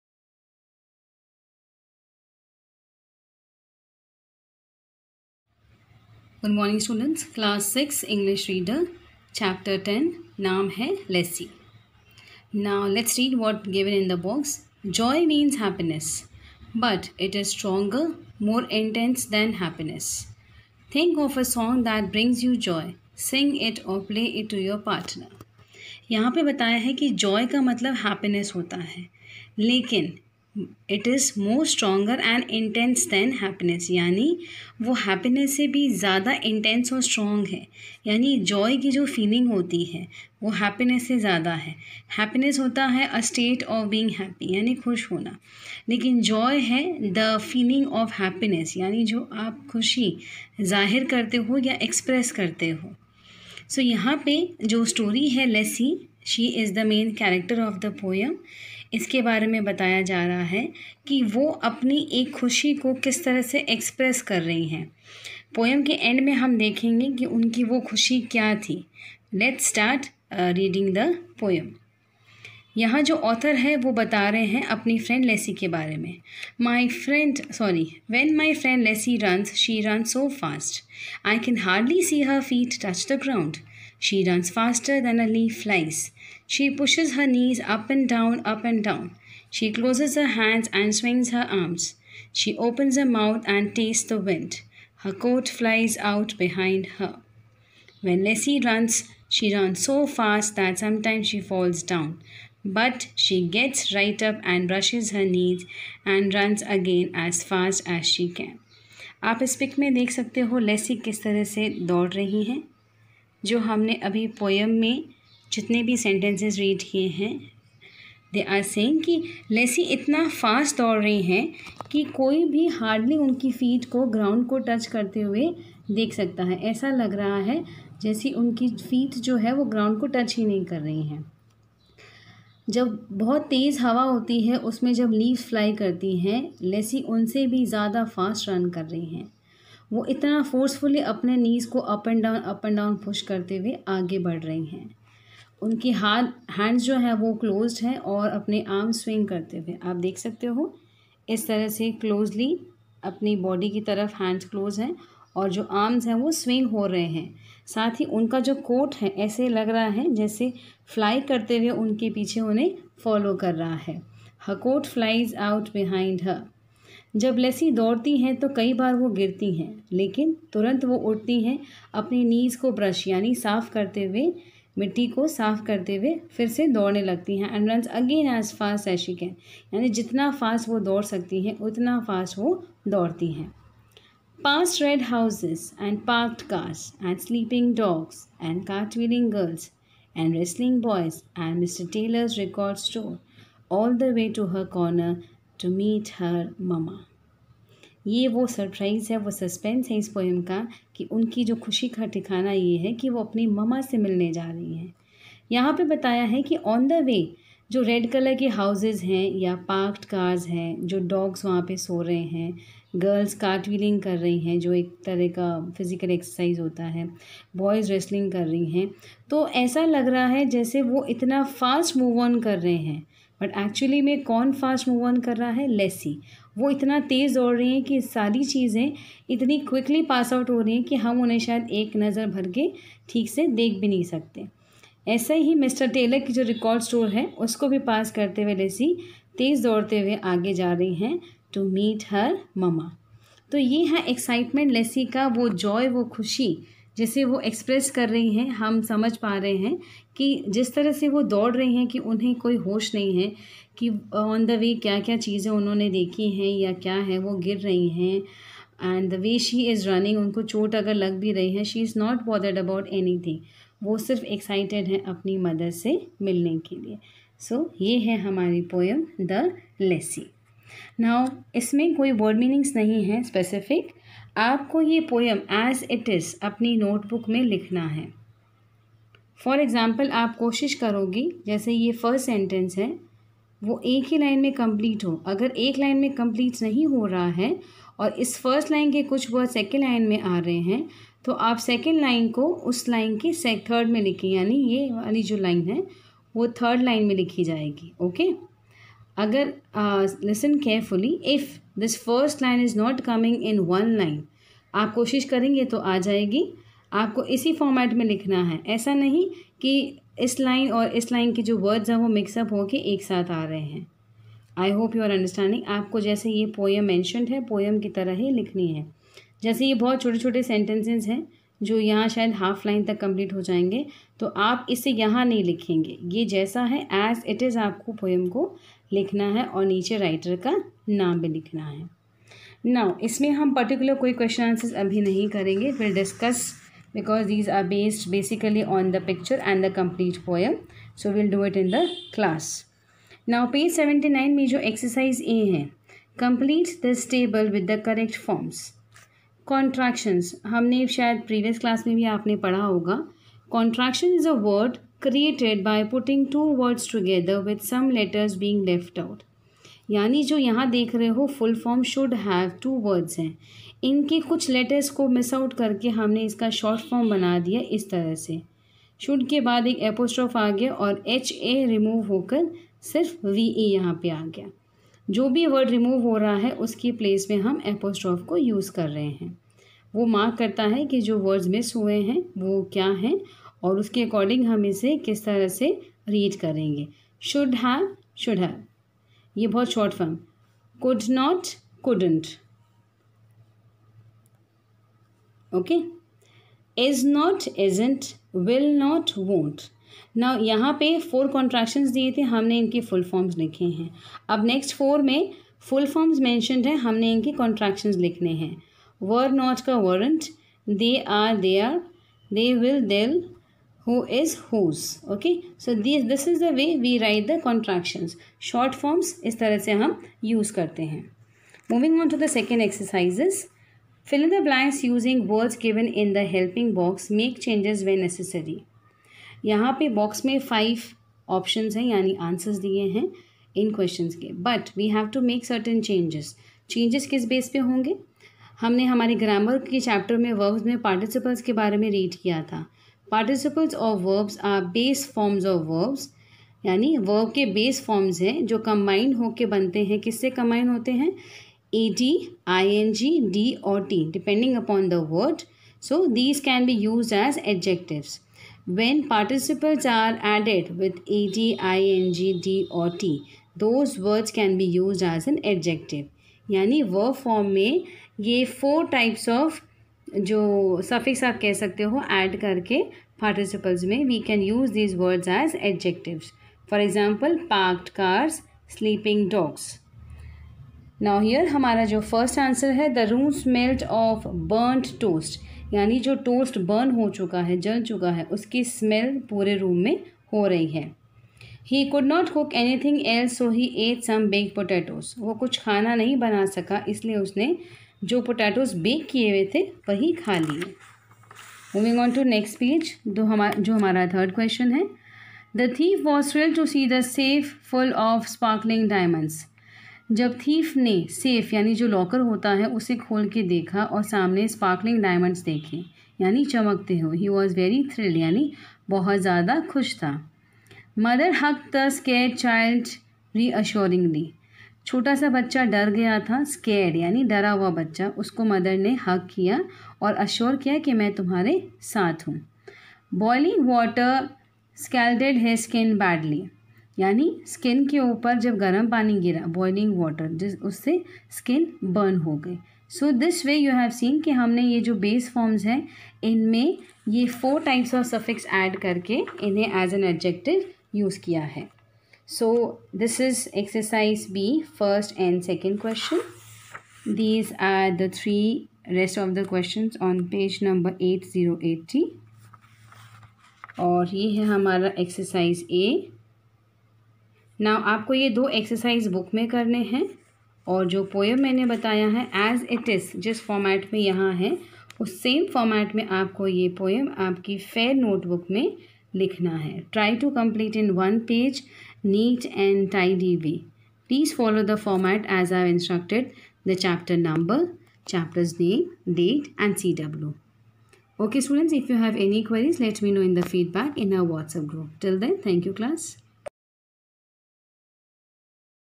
Samta ma'am from SPS ( Suraj Public School)
Class 6 English Reader chapter 10 .Lessie. Part 1